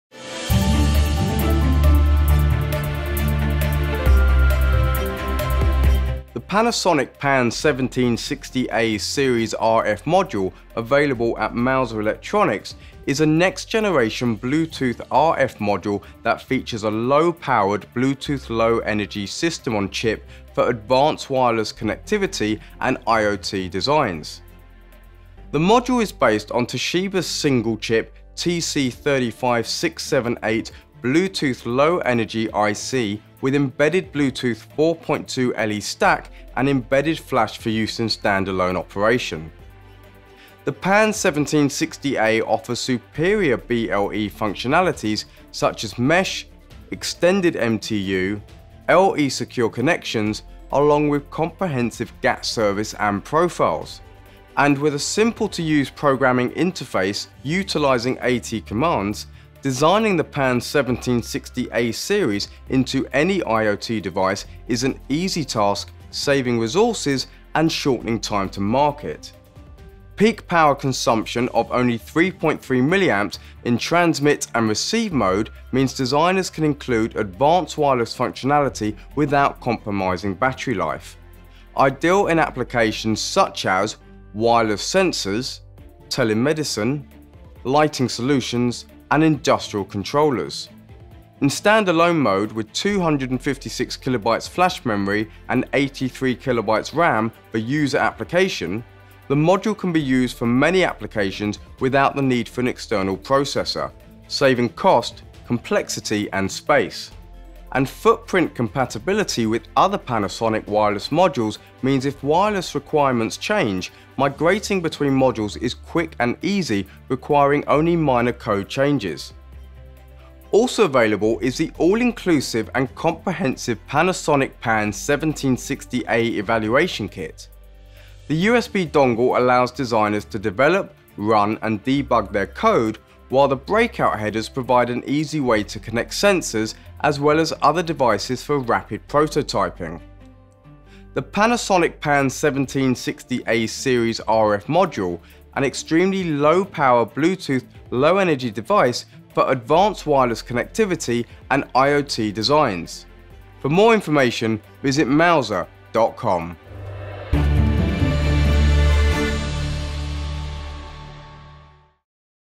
Panasonic PAN1760A Bluetooth Low Power Module - Featured Product Spotlight
Panasonic PAN1760A Bluetooth Low Power Module is a next-generation Bluetooth® RF Module that features an extremely low powered Bluetooth low energy System-on-Chip (SoC) for advanced wireless connectivity and IoT designs.